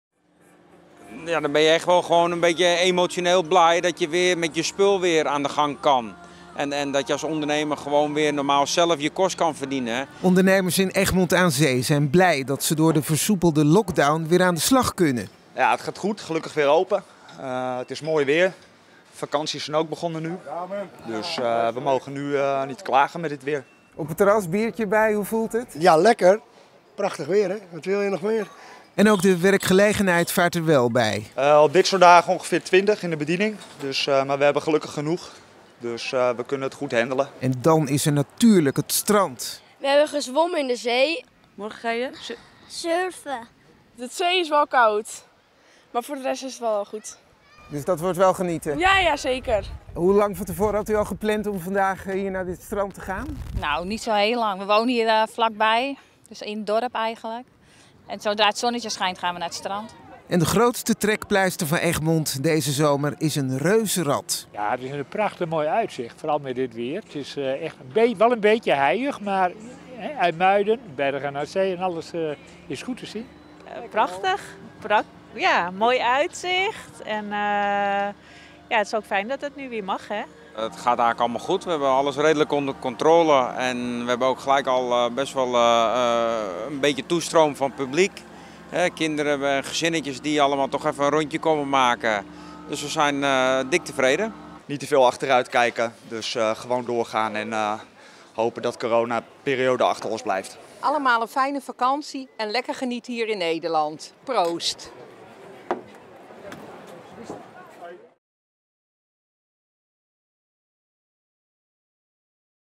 Ondernemers in Egmond aan Zee aan het werk: "Ik ben echt emotioneel blij"
Het was van het weekend genieten op het strand in Egmond aan Zee. Strandgangers nemen een duik, liggen lekker op het strand of nemen een drankje bij een van de strandtenten. Ook de ondernemers in het kustdorp zijn blij nu ze weer mogen werken. Vooral Jan Vallentgoed, die eerder al voor de camera verscheen.

Jan Vallentgoed ziet met trots zijn Europa Rad weer draaien. Met klanten erin. "Ik ben echt emotioneel blij dat ik weer aan de gang kan. En dat ik als ondernemer weer normaal zelf de kost kan verdienen", vertelt hij mediapartner NH Nieuws. Een oudere man die net uit het reuzenrad stapt, is zeker niet teleurgesteld. "Een prachtig mooi uitzicht, vooral met dit weer. IJmuiden, Bergen aan Zee... alles is goed te zien."

Melle van Vliet van de strandtent Nautilus is ook verheugd. "Het gaat goed, gelukkig zijn we weer open. Ik heb 20 mensen in de bediening, dus we kunnen het goed aan. Niet teveel achteruit kijken en gewoon doorgaan. Het is te hopen dat de coronaperiode achter ons blijft."

Strandgangers liggen op strandbedden, maken een wandelingetje of doen een strandtent aan. Zo nu en dan gaat iemand het water op of in. "De zee is wel koud", vertelt een meisje na een duik in de zee. Een vrouw liet weten dat ze wel vaker op het strand te vinden is: "We wonen dichtbij, zodra de zon schijnt gaan we naar het strand."